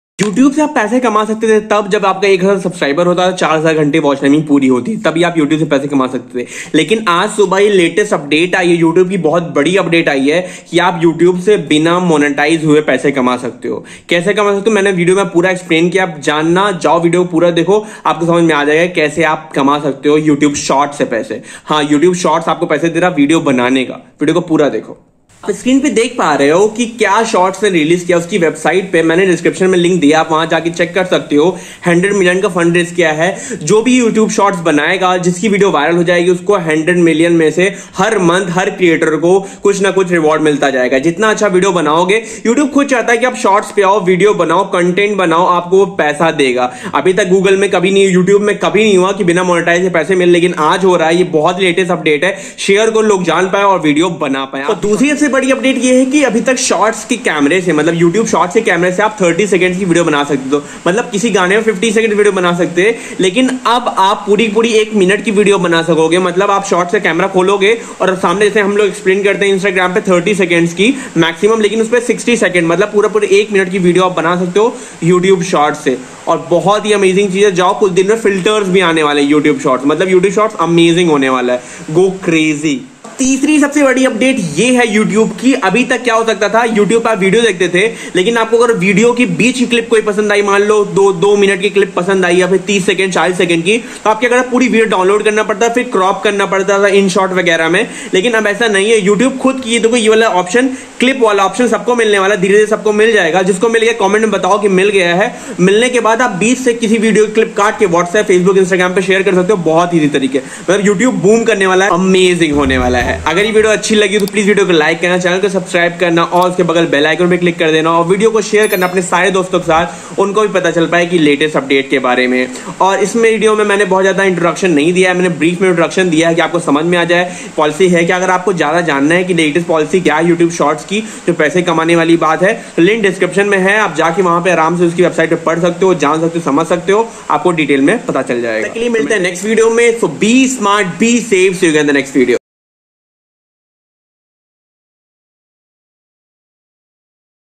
EARN MONEY WITHOUT MONETIZATION || YOUTUBE SHORT 3 MAJOR LATEST UPDATE 😍 || BEST EVER UPDATE
Hello Friends,
In this video I'm going to tell you 3 MAJOR update about YouTube Shorts. That how you can earn money through it without monetize your channel because 1000 followers and without 4000 hours watch time.
Only you can earn money through making only YouTube Shorts.
In youtube new update YouTube release $100M fund for youtube short for creators who make video on Youtube shorts if your short got good engagement You will earn money through it.
If you want to know more about new update.

YouTube Shorts official link - -